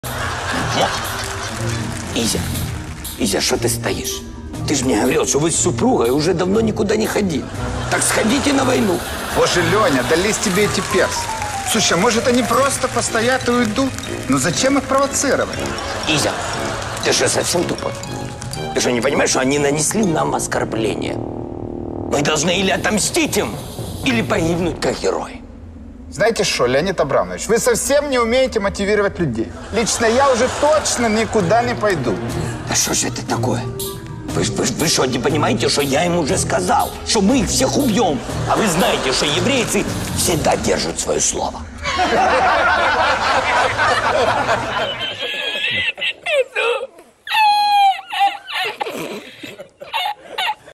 Изя косит от войны.
Воинственный Леонид Абрамович, Царь древней Хайфы, не оставляет надежду собрать войско против Персов.

В ролике использованы материалы с Ютуб-канала @studiya95kvartal